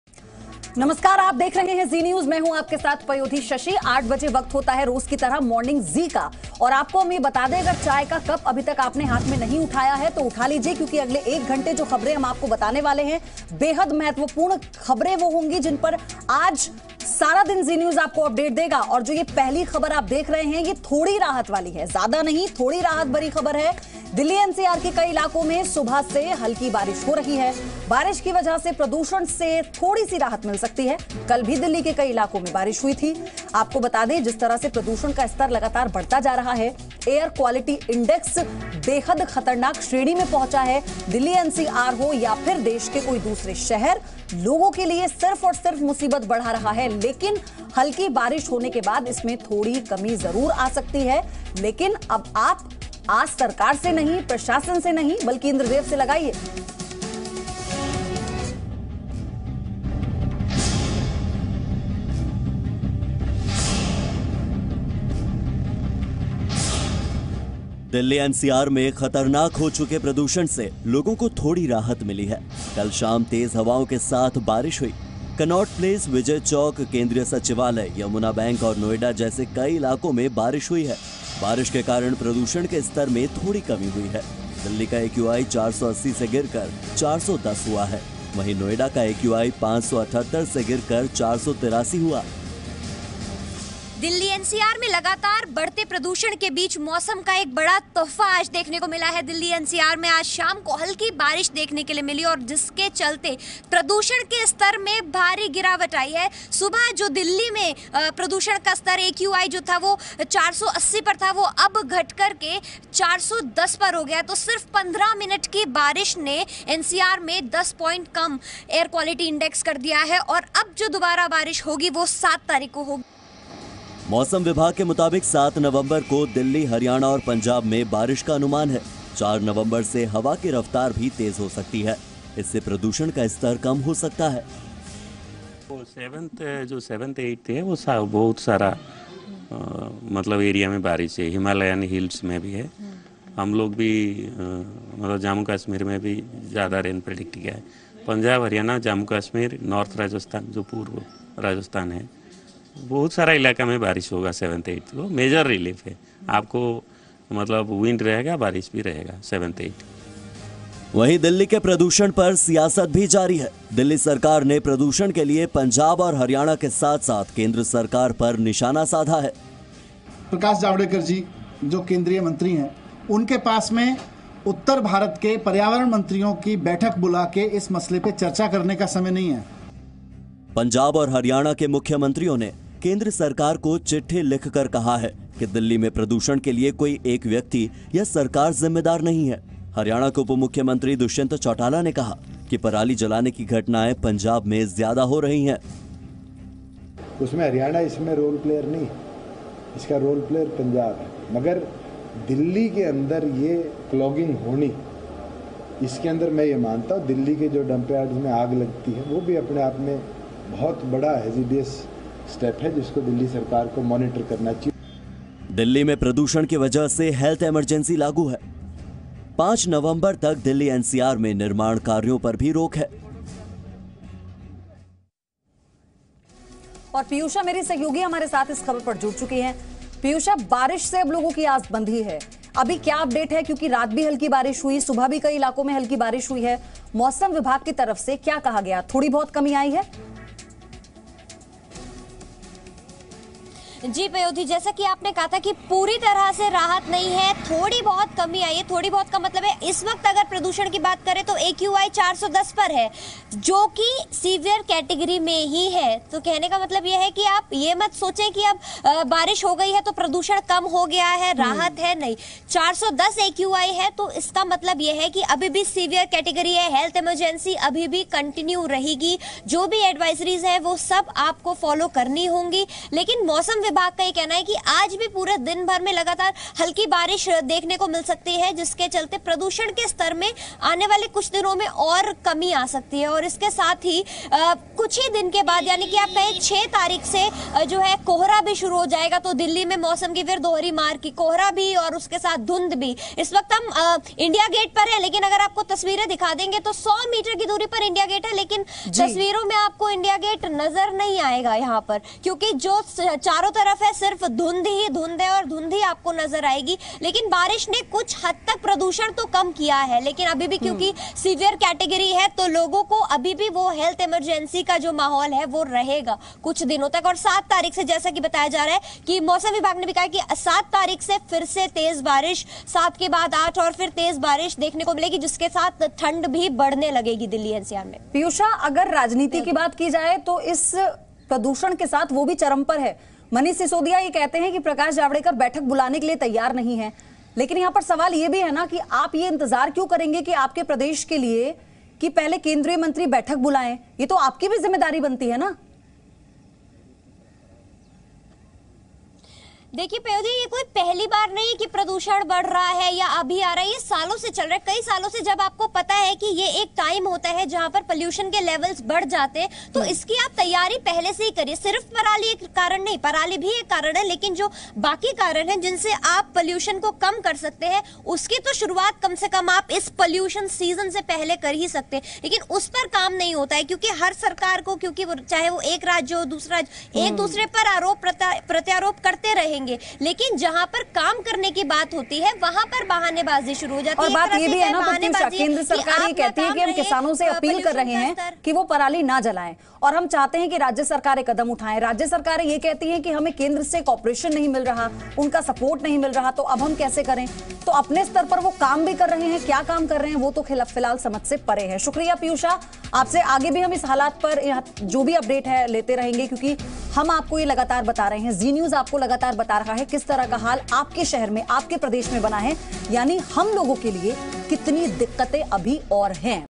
Decline in pollution in Delhi-NCR due to rain, expect some relief
The Decline has been recorded in pollution in Delhi-NCR due to rain, expect some relief. Watch this video to find more in detail.